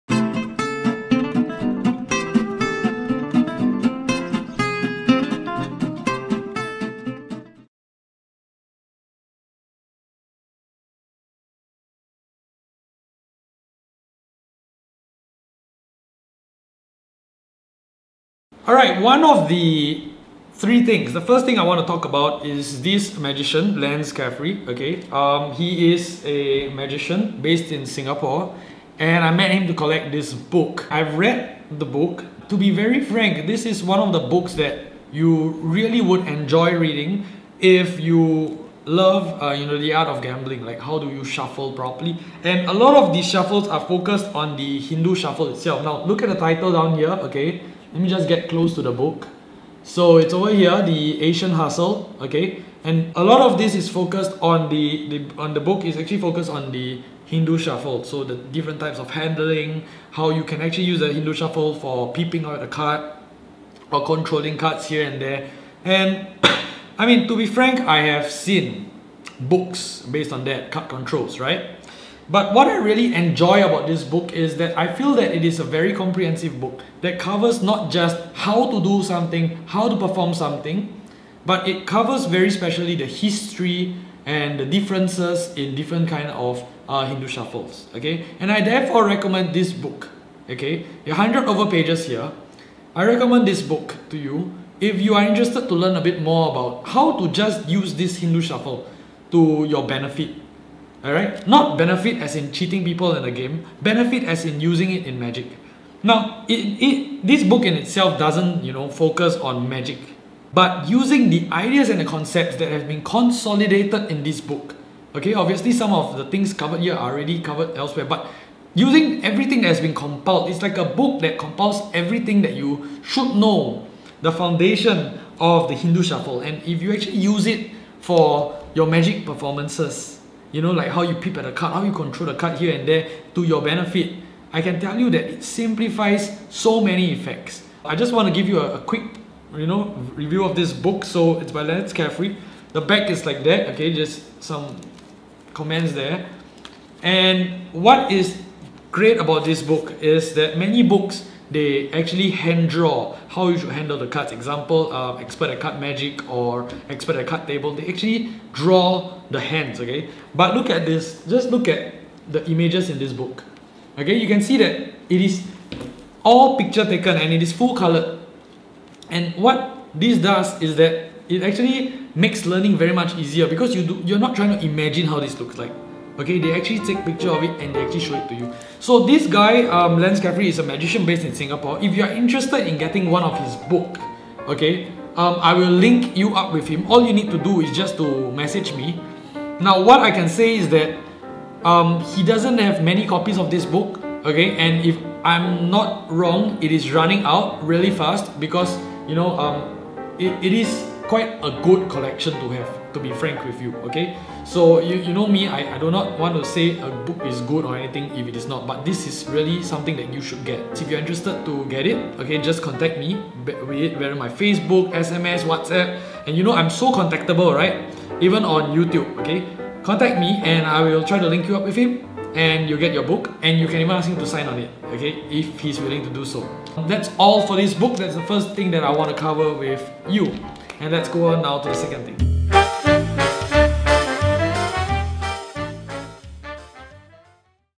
Magic Desu : Book Review The Asian Hustle - Secrets of the Hindu Shuffle
Here I look in to one of the latest books in the magic world. Written by our very own local magician, this book is like the full package of what you will want to know about the Hindu shuffle!

I will also show you a sneek peek of what the inside of the book looks like!